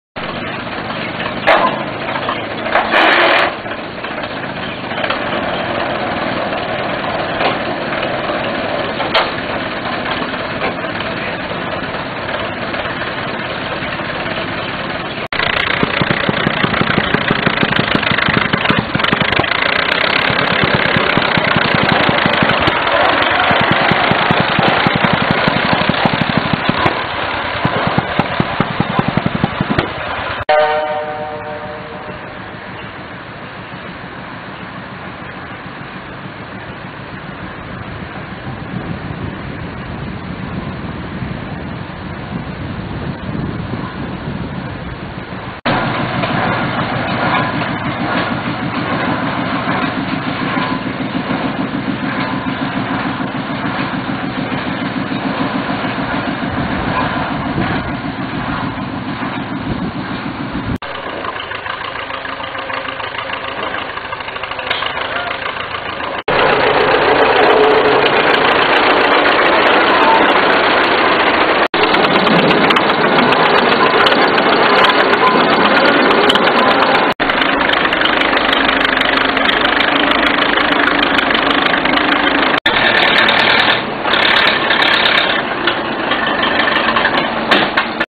TACL Rail Tractor
This is the TACL Rail Tractor when it was still in one piece many years ago. Sadly it's now in bits. The TACL ran on a timber tramway off Collins Siding on the Moe-Walhalla VR narrow gauge.